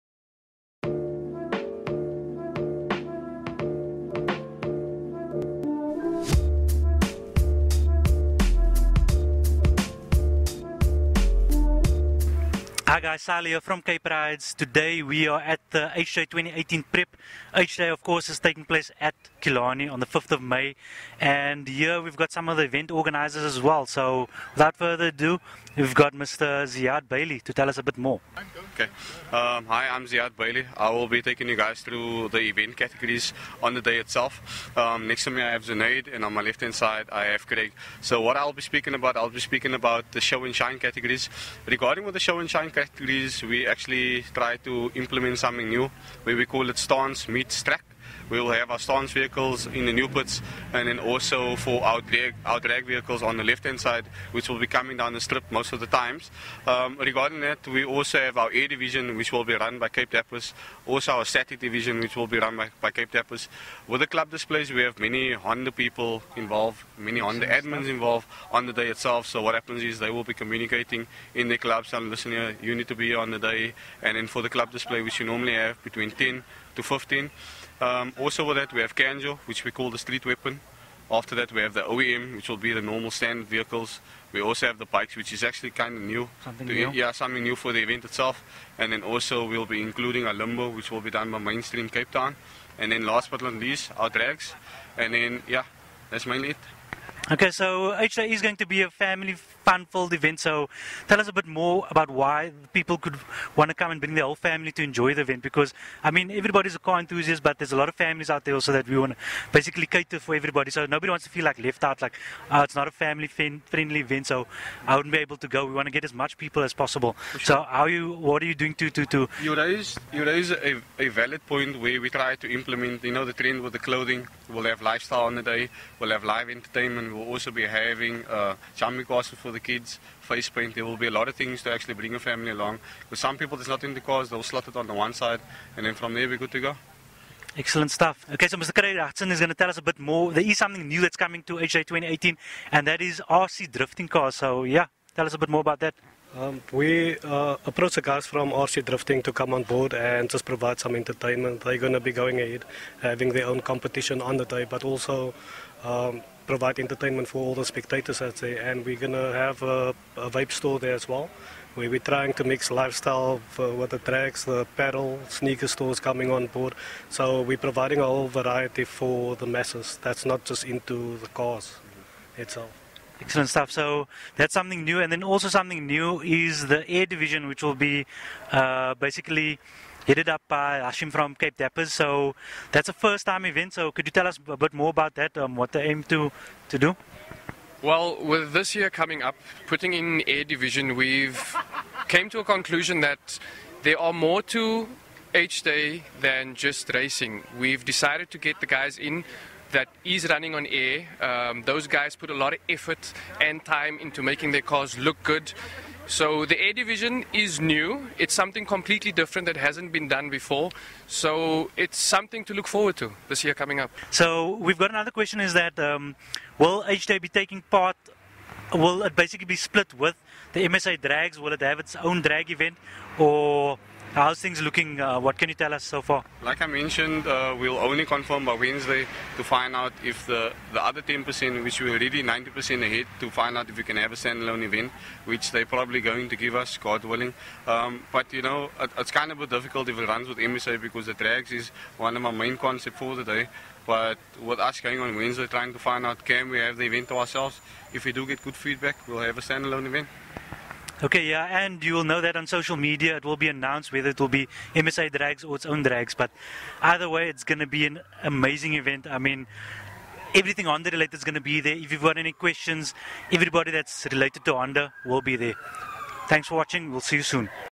HDAY Cape Town 2018 - Organizers
A few words from the organizers of this years HDAY Cape Town 2018 event taking place on 5th May at Killarney Raceway. Be sure to be there!